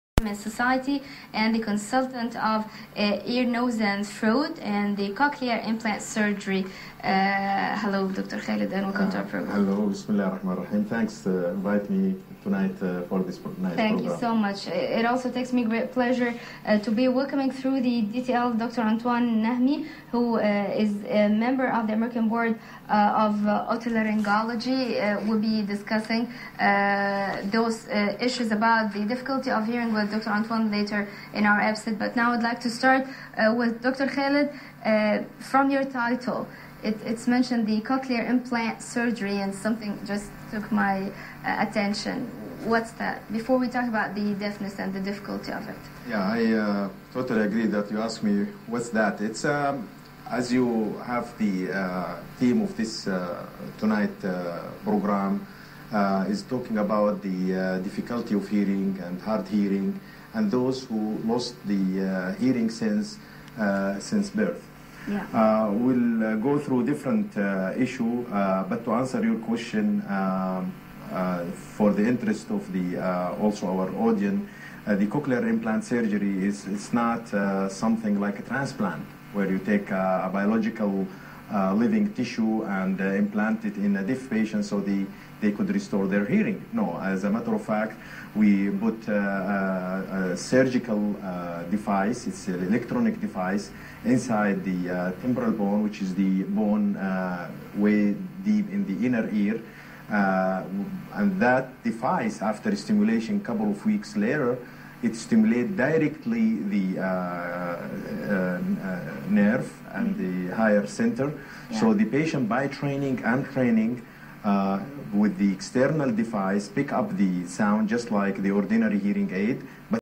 لقاء مع الدكتور خالد طيبة Cochlear implants for the deaf
لقاء عن زراعة القوقعة بالأنجليزي ، Cochlear implants for the deaf